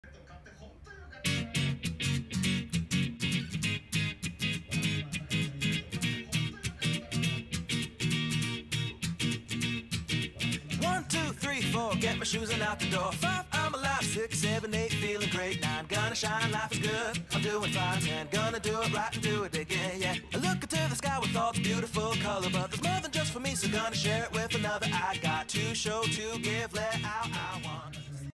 うちの猫 04 My Cat 04 総一Movie
総一の成長動画日記です。
SOUICHI's growth movie.

2015年春から年中さんです
幼稚園楽しいです。 運動会とお遊戯会も終わって年中さんもあと少し
5歳です。5years old